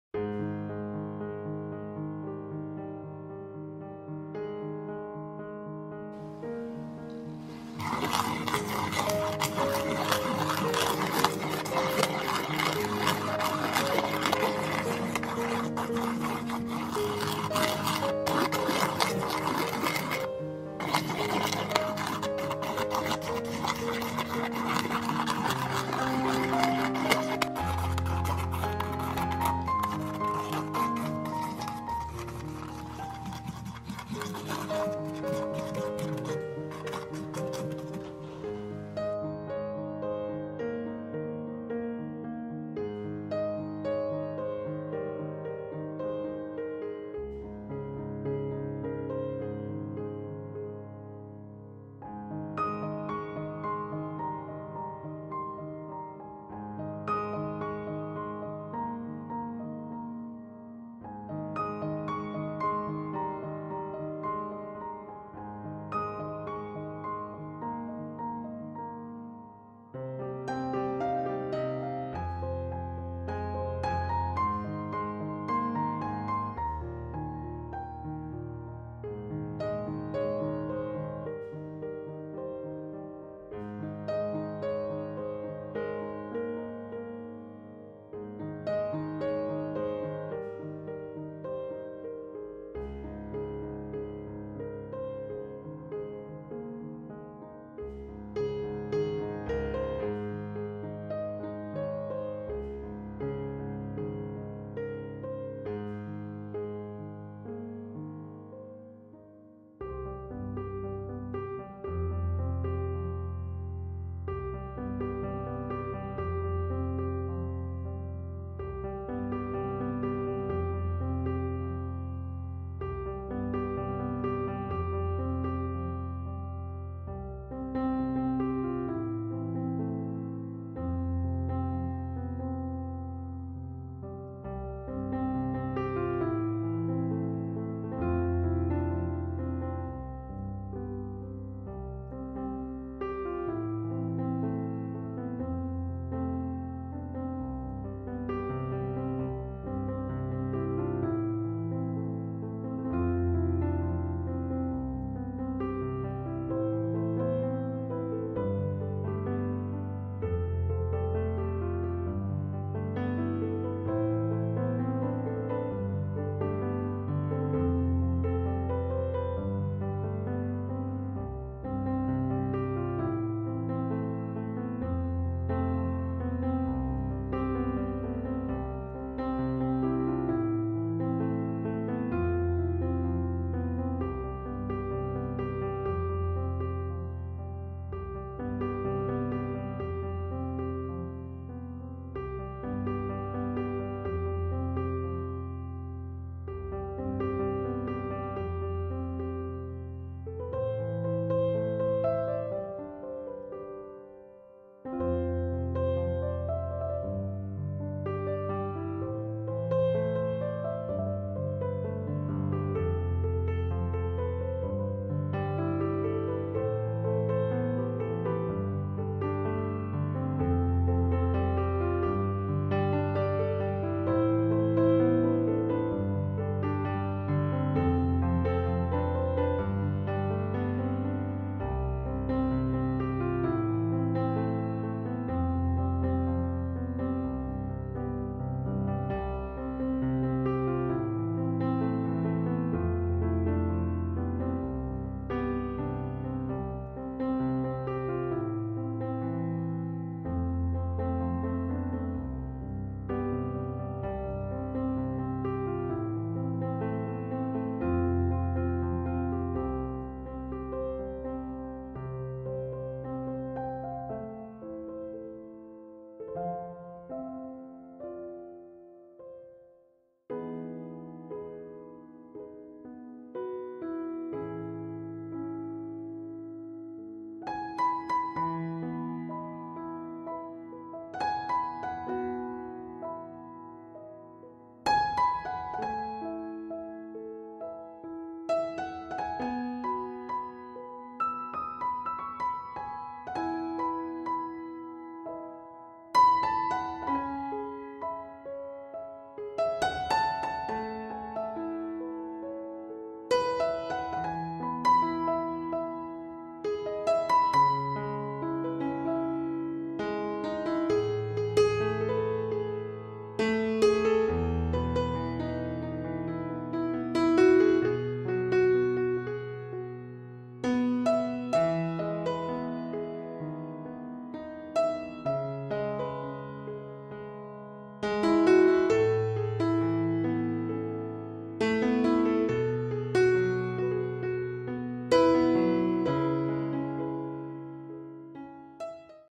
우주 고래 그리기 / Color pens & Acrylic Painting #13
사용한 재료 :
리퀴텍스 베이직 아크릴물감
모나미 수성 플러스펜 48색

Material :
Liquitex basics Acrylic paint
Monami water-based plus pen 48 Color